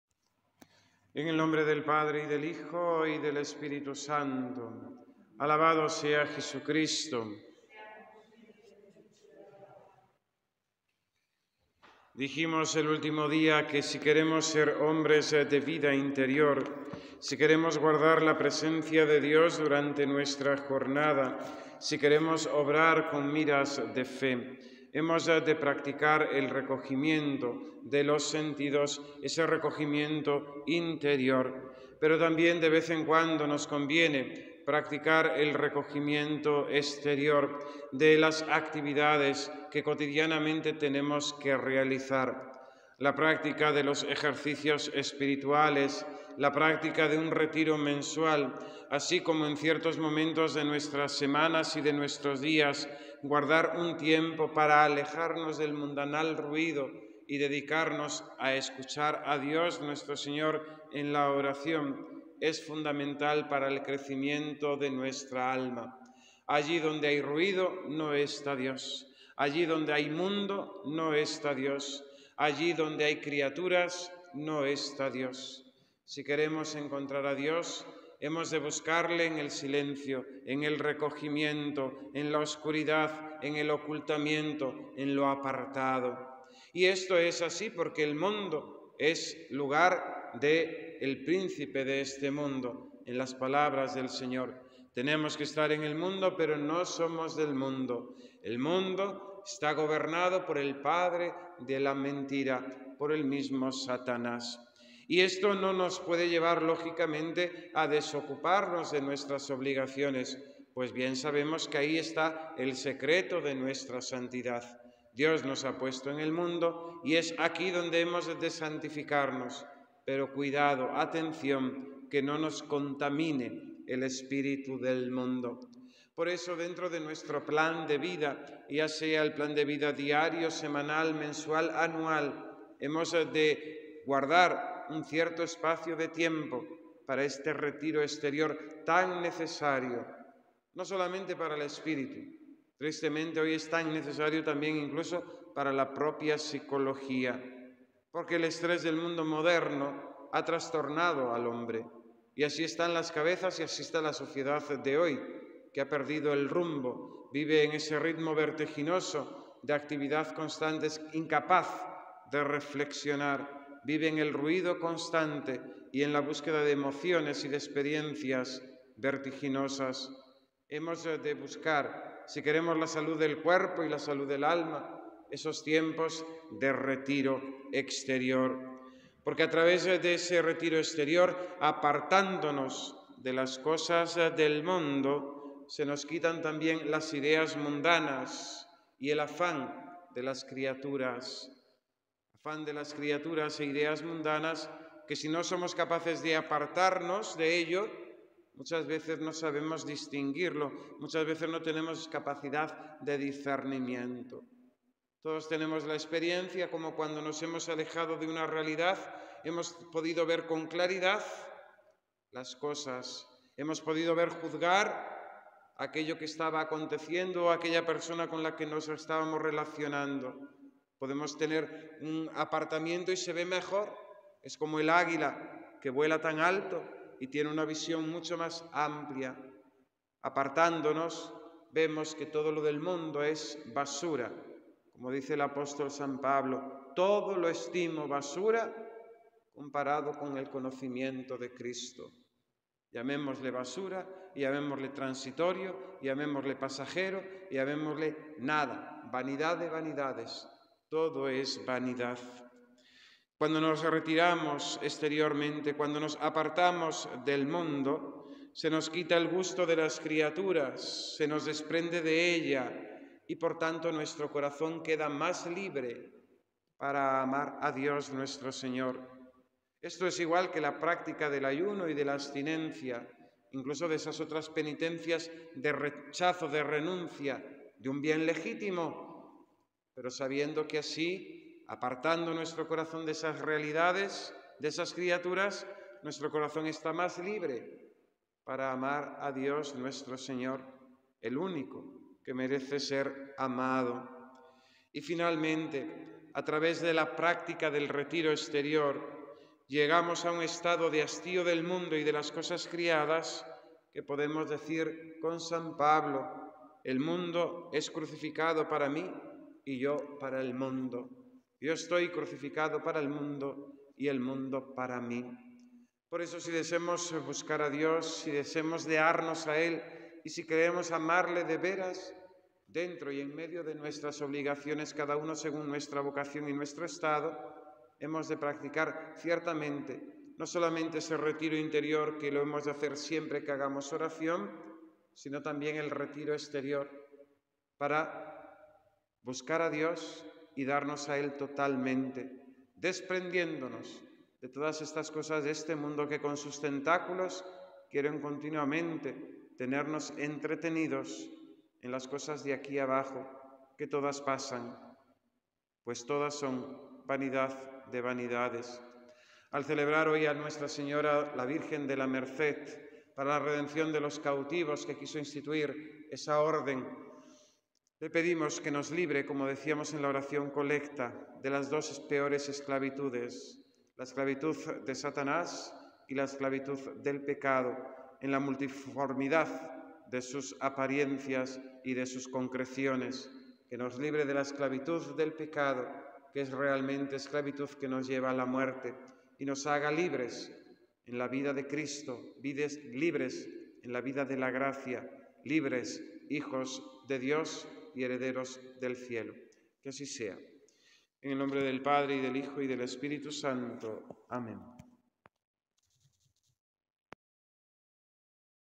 EL RECONOCIMIENTO EXTERIOR. MEDIOS PARA SER HOMBRES DE VIDA INTERIOR. Homilía
Doctrina espiritual de san Juan Bautista de la Salle y san Antonio María Claret